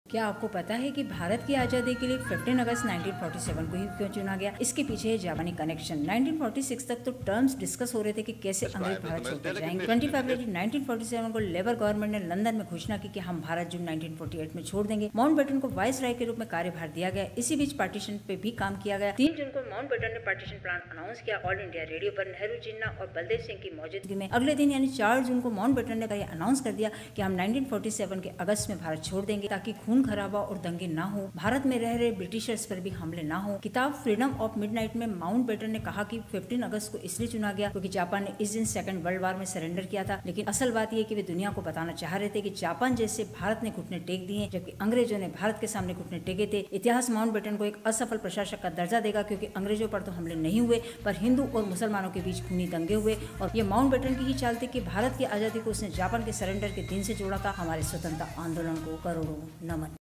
Why August 15, 1947 as Independence Day 🔍
Dive into the deep-rooted significance of August 15, 1947, in India's journey to independence. Uncover the historical context, pivotal decisions, and intricate narratives that shaped this momentous day. Explore the untold stories behind India's freedom struggle and its connection to global events. Discover the multi-faceted reasons behind the chosen Independence Day date. Join us on a captivating historical journey of insights and revelations. 🇮🇳